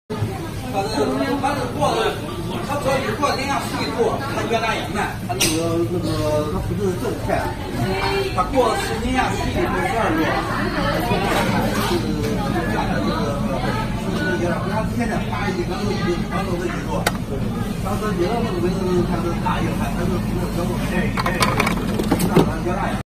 Mini black/White chocolate enrobing coating tempering machine with cooling tunnel
We are the factory of mini chocolate enrobing machine with CE. 8kg, 15kg,30kg,60kg for your choice. We supply: OEM service.
Wechat:Amy123vicky